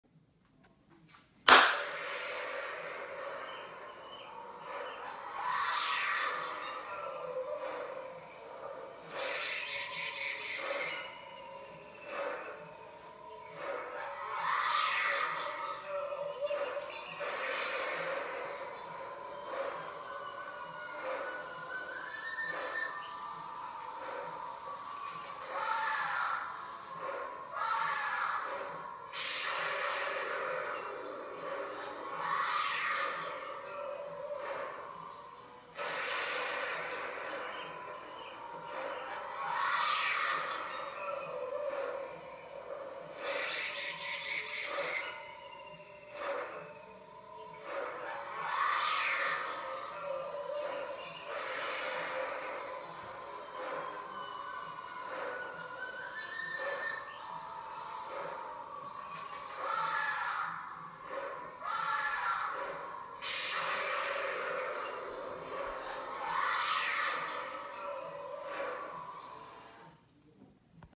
Scary Flying Monsters - Vampire Bat
This giant Vampire Bat actually “flies” along a 25 ft. Cable! The Bat’s wings flap around as it emits creepy sounds and it’s eyes flash blood red. Sure to be a hit at your Halloween party🦇

Made by Tekky Toys in 2005